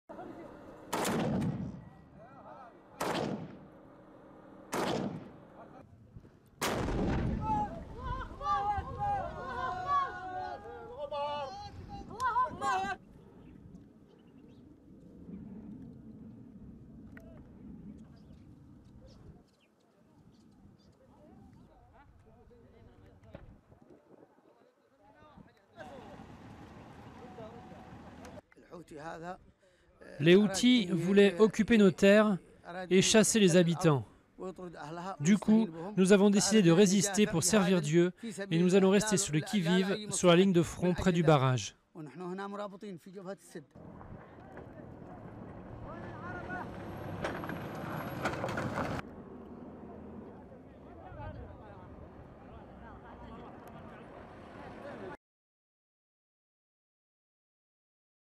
Yémen: combats pour le contrôle du barrage de Marib
Au Yémen, des combats pour le contrôle du barrage de Marib, contrôlé par les rebelles, ont eu lieu jeudi 17 septembre 2015, avec la participation de combattants de tribus sunnites alliés aux forces progouvernementales. Durée: 00:58